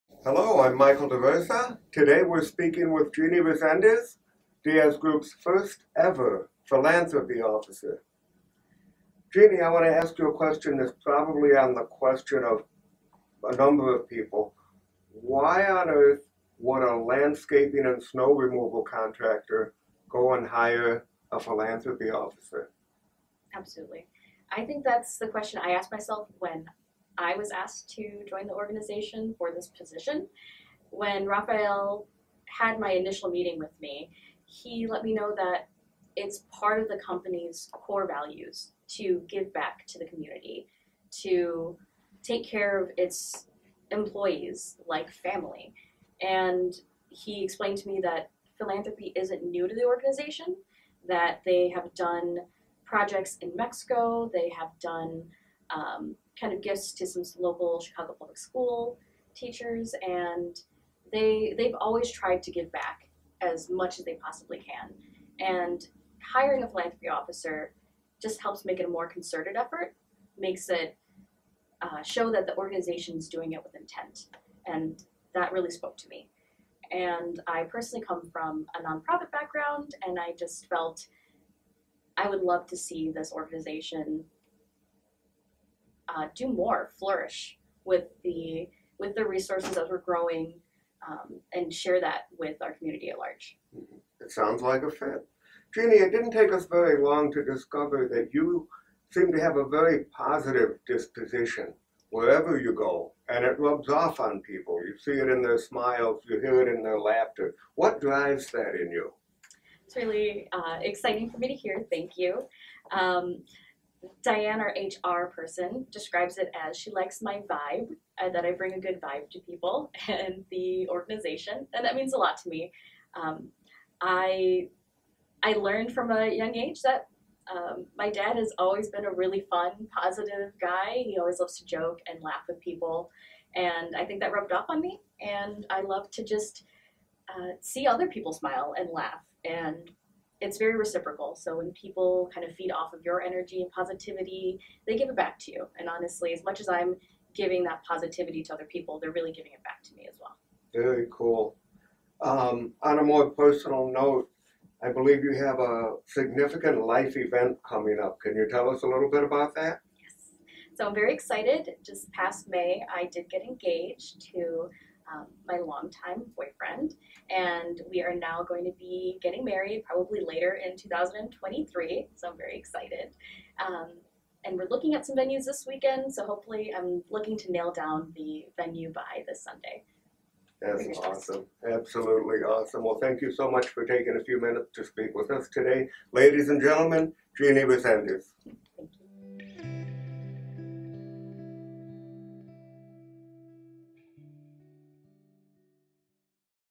Diaz Group Employee Spotlight: Jeanie Resendiz
The term “philanthropy” dates back to ancient Greece and has come to refer to generosity in all its forms, i.e., time, treasure, and talent, for the welfare of others. Diaz Group LLC has engaged in philanthropic outreach for years as a natural means of putting its core values into practice. We have now formalized this aspect of Diaz Group and ask you to join us in welcoming Jeanie Resendiz to our family in the newly created role of Philanthropy Officer.